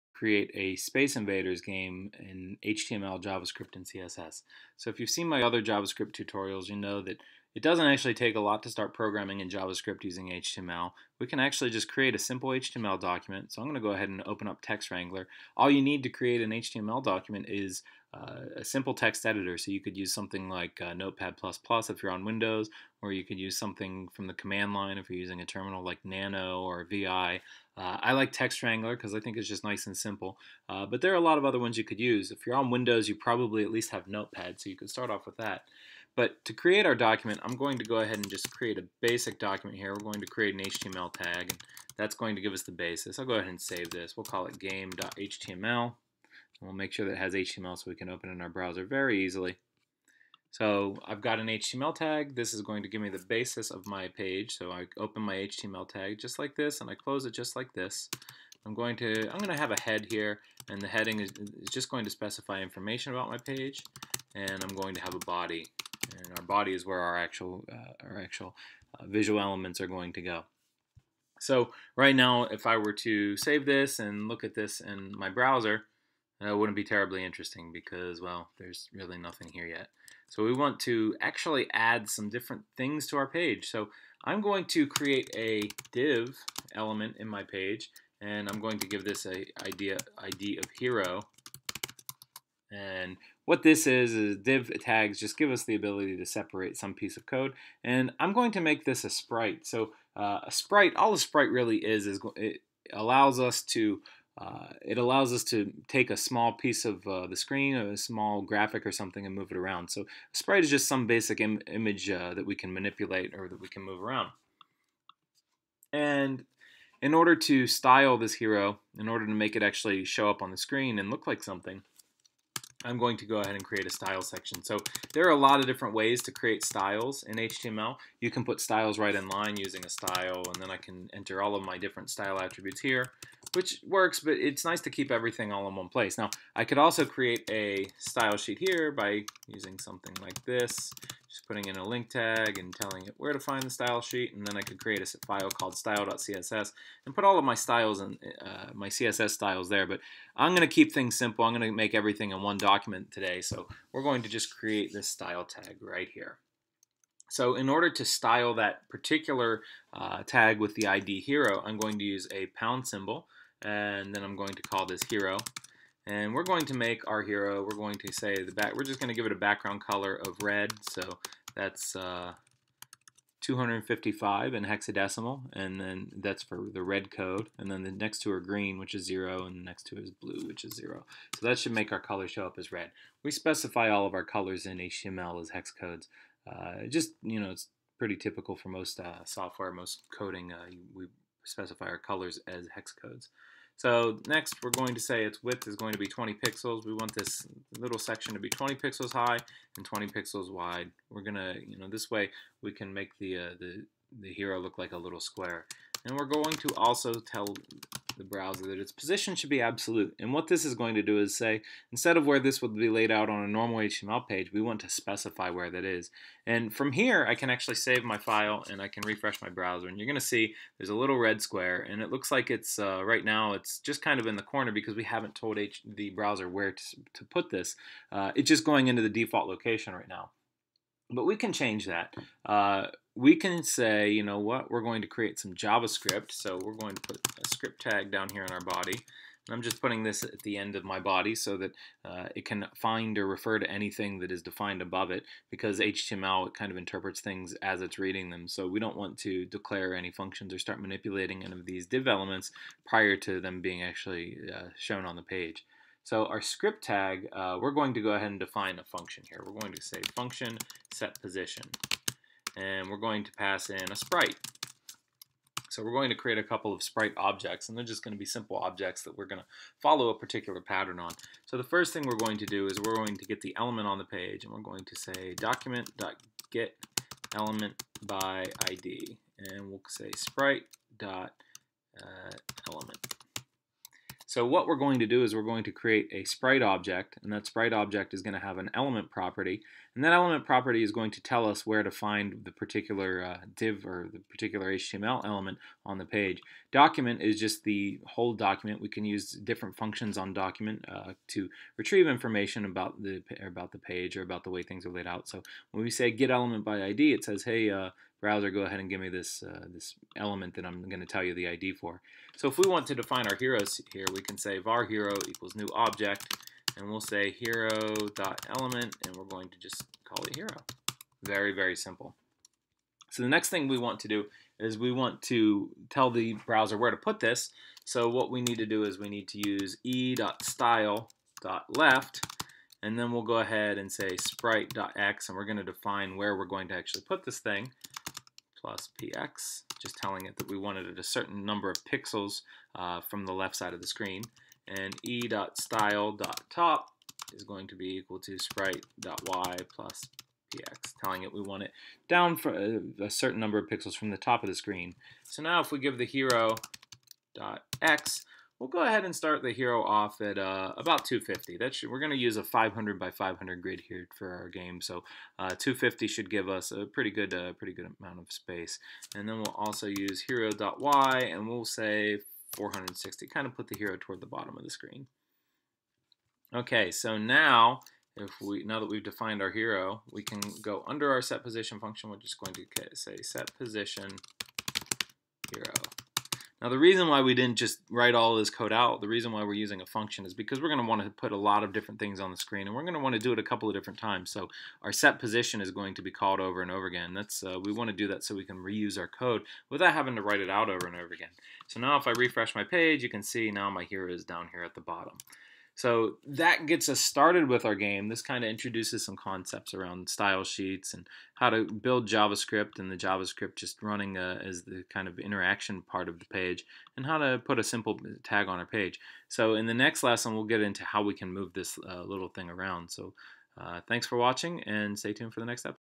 Learning HTML and JavaScript with a Simple Shooting Game: Lesson 1
This is a simple exercise meant to teach teens and beginning programmers the basics of programming. This lesson shows how to start interacting with the page with Javascript and styling the page using CSS.  You can find the code for this lesson

and check out for more coding tutorials!

If I've helped you learn to code in 2017, please go and let me know!